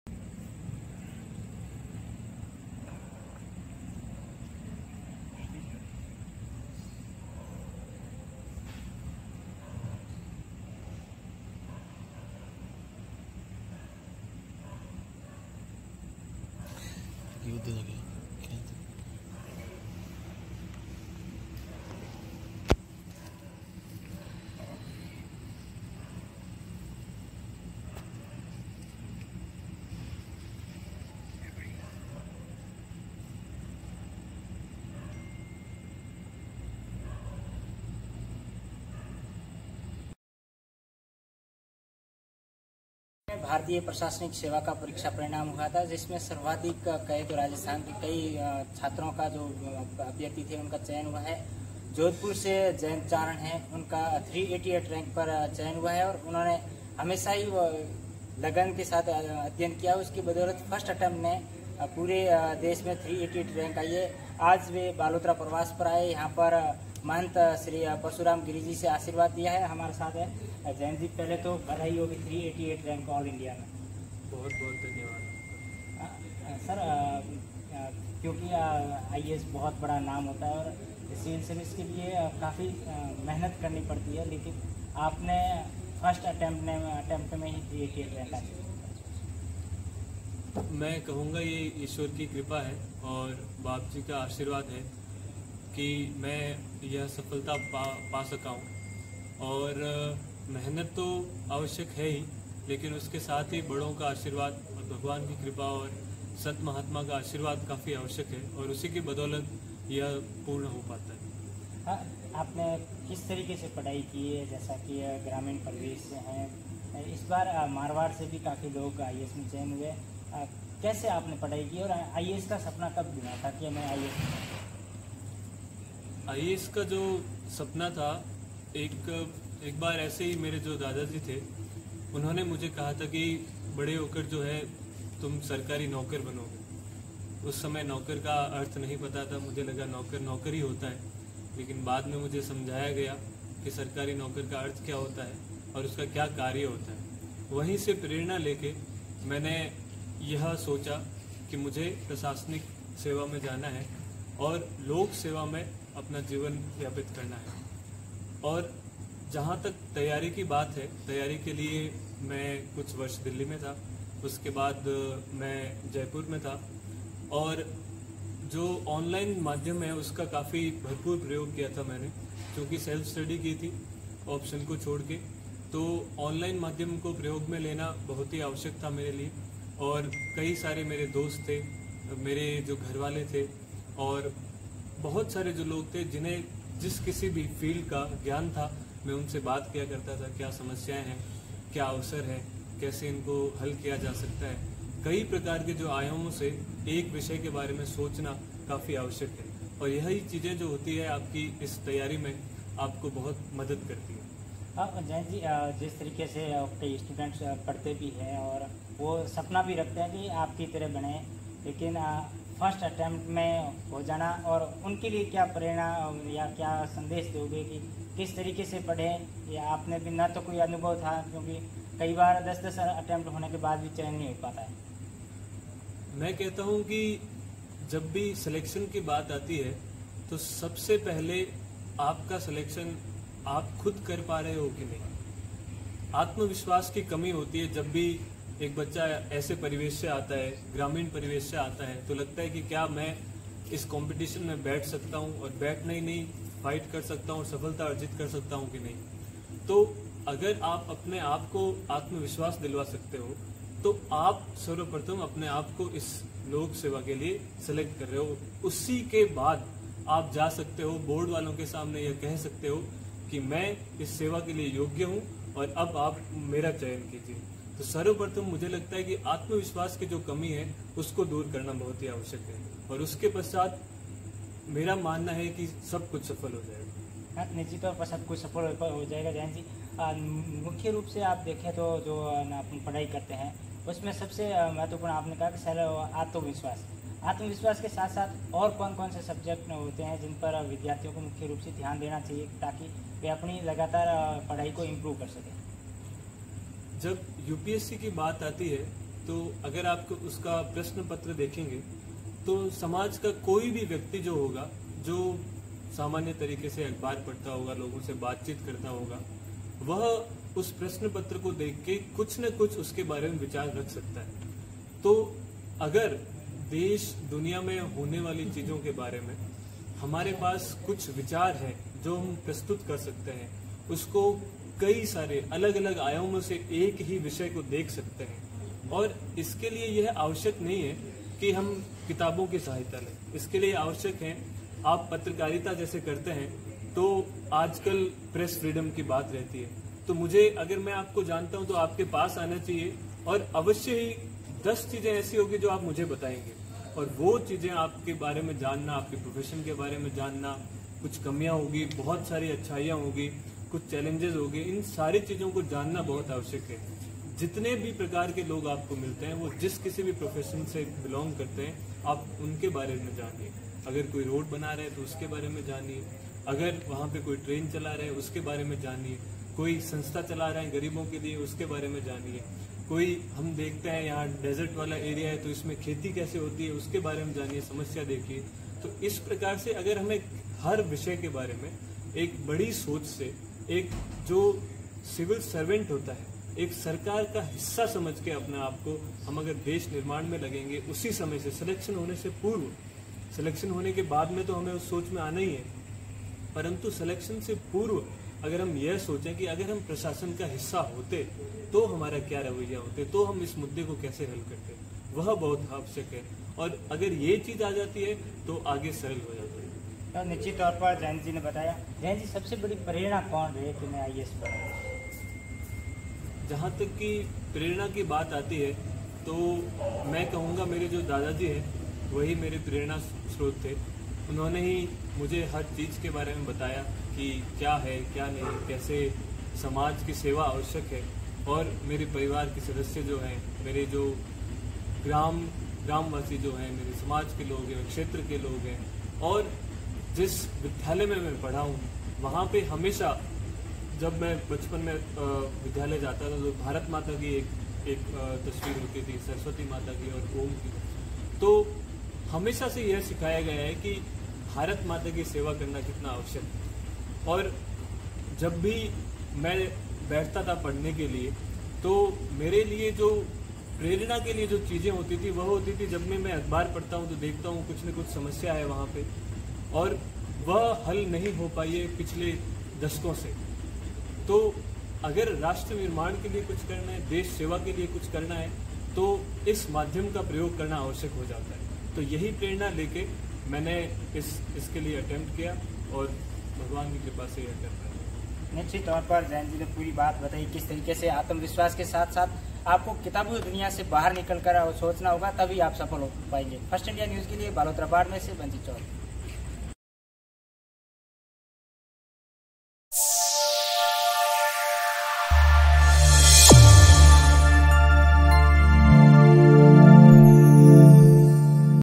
पहले प्रयास में ही किया है UPSC क्रैक, कनाना पहुंचने पर मठ में किया गया जयंत का स्वागत | Barmer News
About Video:

पहले प्रयास में ही किया है UPSC क्रैक, कनाना पहुंचने पर मठ में किया गया जयंत का स्वागत | Barmer News | 3 June 2023